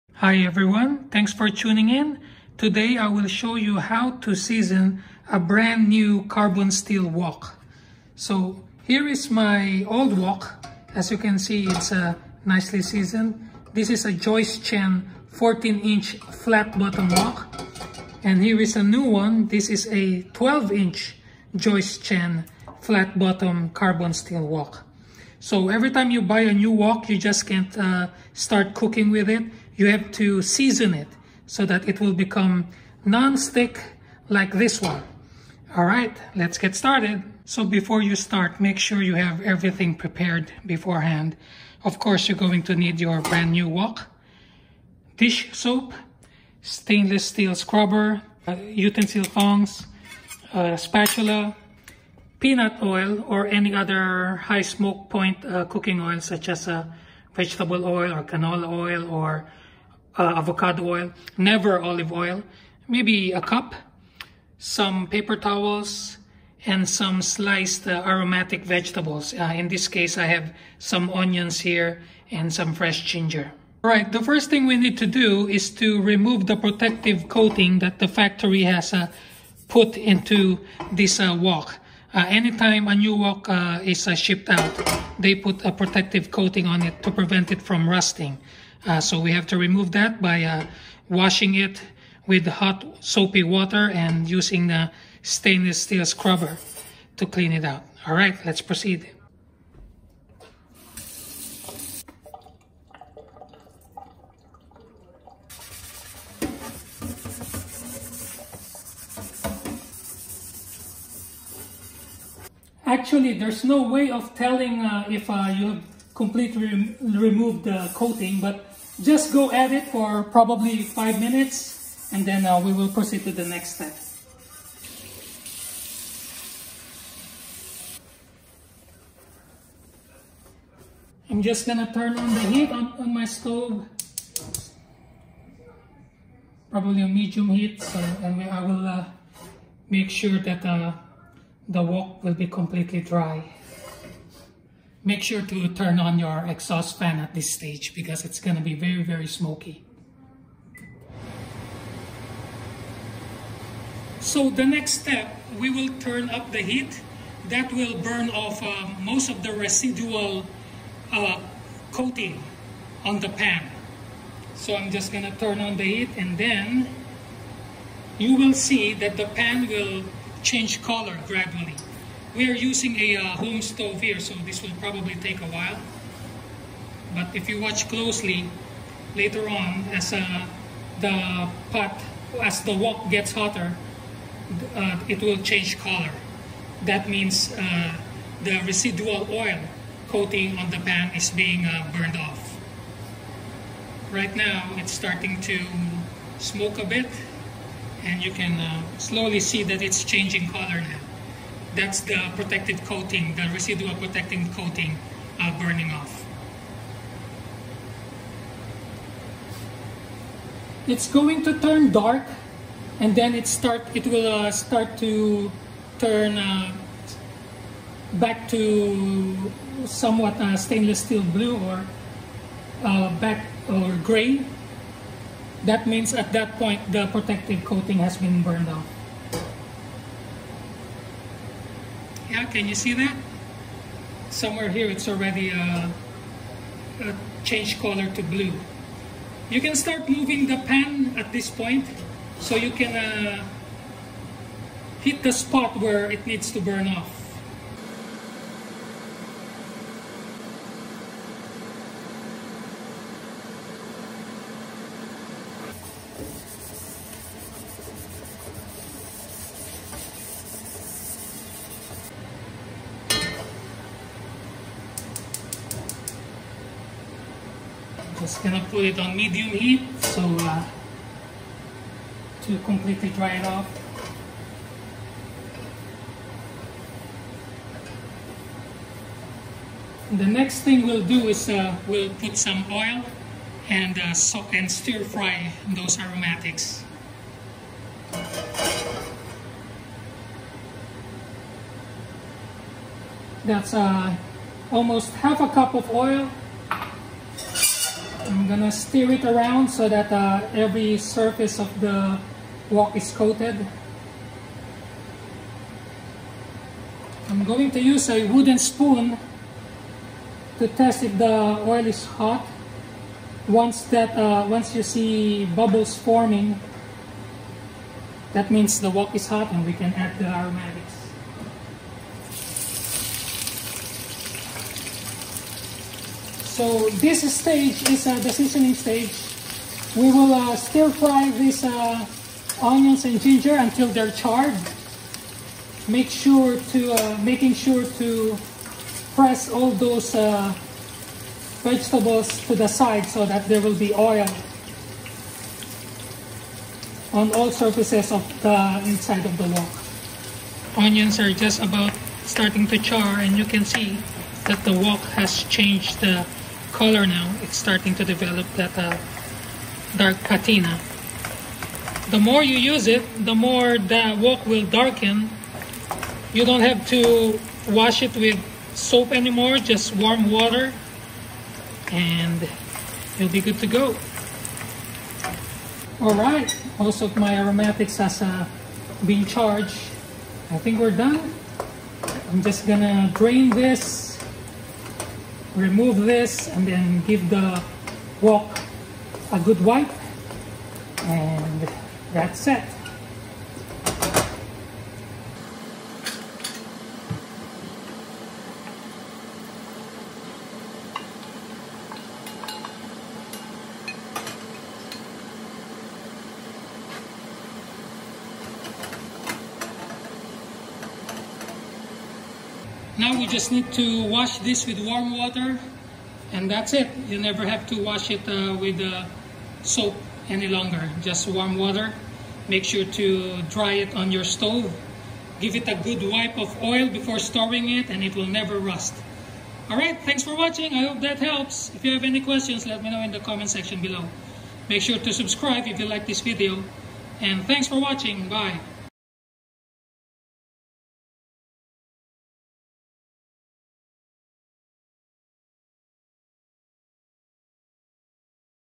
How to Season a Carbon Steel Wok ⎮Joyce Chen 12 inch Wok.
Where to buy
This is a video on how to season a carbon steel wok.  Step-by-step instructions listed below.

I am using a Joyce Chen 12" Flat Bottom Carbon Steel Wok.  A new wok needs to properly seasoned before using for cooking.  This will ensure that the wok will have a non-stick surface.

You will need a wok, dish soap, stainless-steel scrubber, paper towel, high smoke point oil like peanut oil, cooking spatula and thongs, some sacrificial aromatic vegetables such as onions or ginger or garlic or spring onions.

1.  Wash the wok using hot soapy water.  This is to remove some of the protective oil coating that the factory has coated the wok to prevent it from rusting.  Rinse thoroughly, then dry with paper towel.
2.  Place the wok on a burner and turn the flame to medium to ensure that all of the remaining moisture in the wok is removed completely.
3.  Put flame on high.  The wok surface will change color.  This is the residual coating being burned off.  It will turn dark, then to somewhat blue steel color.  Keep turning the wok to burn off other spots so that the wok will be completely coating-free.  You can tell this by the color change.
4. Once the burn-off is complete, let the wok cool down a bit, or if you are brave enough, pour water into the wok to cool it down, then gently wash with hot water and soft sponge to remove any residual debris.  Rinse and wipe dry.
5.  Return wok into stove and turn on the heat once more to completely dry the wok.
6.  Pour a half cup of cooking oil, peanut oil if possible, into the wok.  Stir the oil around to ensure that the surface of the wok is fully coated in oil.  Add the aromatic vegetables and stir fry until charred.  This will take around 15 to 20 minutes.  You will notice that the wok will change color again, turning darker as it develops that wok patina.
5.  Once the vegetables are charred, discard them and wash the wok with warm water.  Towel dry and return the wok into the stove, turning on the heat once again to completely dry the wok.

Your wok is now seasoned!  Before storing, wipe it with some oil to prevent it from rusting.  Only wash it with hot water and never with soap to prevent the seasoning from being washed away.  Use a soft sponge to remove any food bits that sticks.  It will take a lot of cooking and use for the wok to develop that dark, non-stick patina, so just keep on using the wok.  Good luck and have fun!